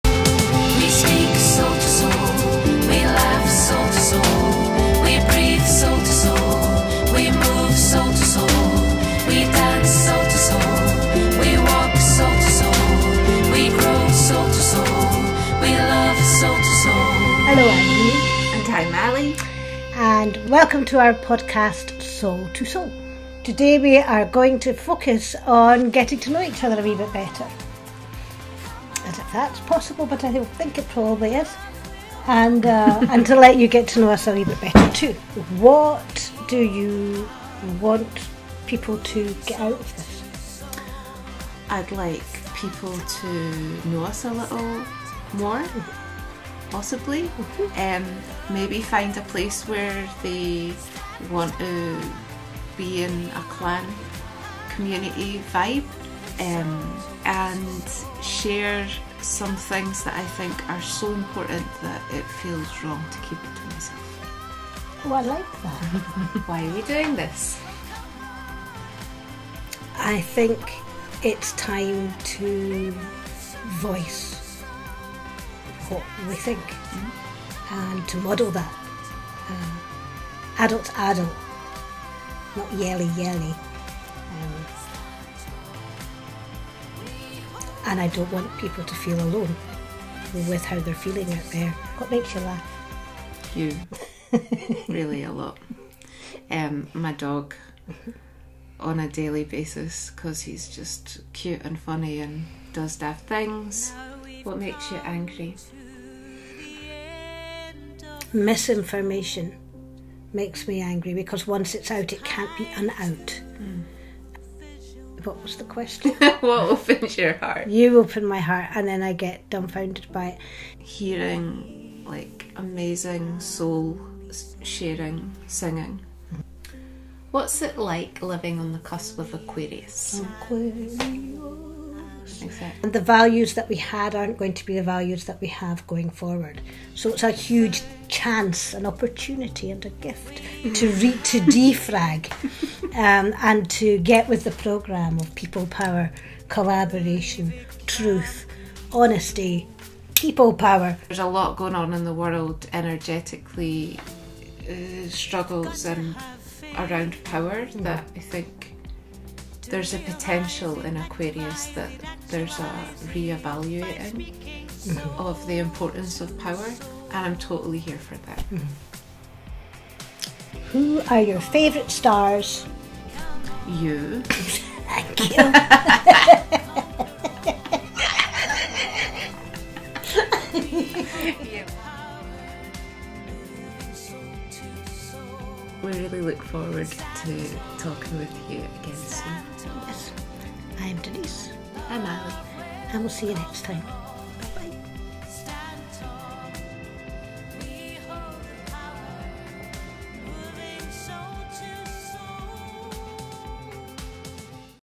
Soul to Soul - Podcast by DnA - Intro
We are super excited to be launching our podcast, Soul to Soul, in a couple of weeks. This wee video gives you an idea of what to expect.